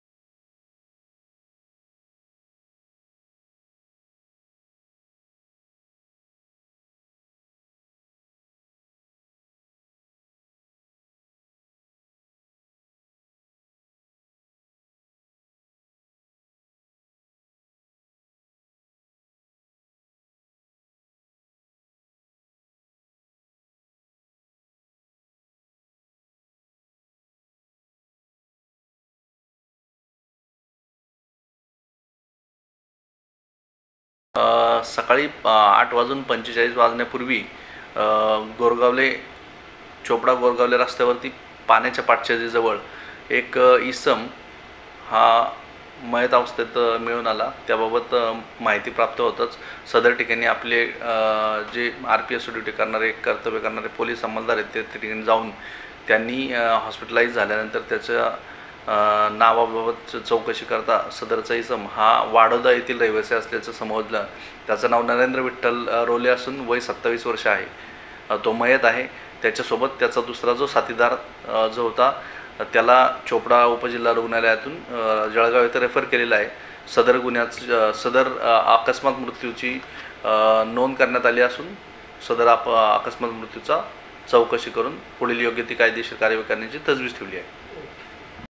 JALGAON | चोपडा गोरगावले रस्त्यावर एक इसम मयत अवस्थेत
चोपडा गोरगावले रस्त्यावर  पादचारी जवळ  एक इसम मयत अवस्थेत  असल्याची माहिती मिळाल्याने पोलीस कर्मचाऱ्यांना त्या ठिकाणी पाठवले असता त्यांनी उपजिल्हा रुग्णालय येथे आणले त्या ठिकाणी त्याची ओळख पटली असतात तो गाडोदा येथील रहिवासी होता त्याच्यासोबत दुसरा साथीदार याला उपजिल्हा रुग्णालयात प्राथमिक उपचार करून जळगाव येथे पुढील उपचारासाठी रवाना केला आहे चोपडा शहर पोलीस स्टेशनला अकस्मात मृत्यूची नोंद करण्यात आलेली आहे पुढील तपास करण्यात येत असल्याचे सहाय्यक पोलीस निरीक्षक अजित कुमार सावळे यांनी सांगितले. सदर या  ठिकाणी नेहमी अपघात होत असतात आतापर्यंत 15 च्या वर नागरिकांच्या जीव या ठिकाणी अपघातामुळे गेलेला असल्याने पादचारी  वरील हा जो पूल आहे. त्याची रुंदी वाढविण्याची वारंवार मागणी करण्यात येत असल्याने तरीदेखील त्याच्याकडे दुर्लक्ष होताना दिसत असल्याचे नागरिकांच्या संताप या ठिकाणी जाणवत होता.

BYTE -  पादचारी  चोपडा शहर पोलीस स्टेशनचे सहाय्यक पोलीस निरीक्षक अजित कुमार सावळे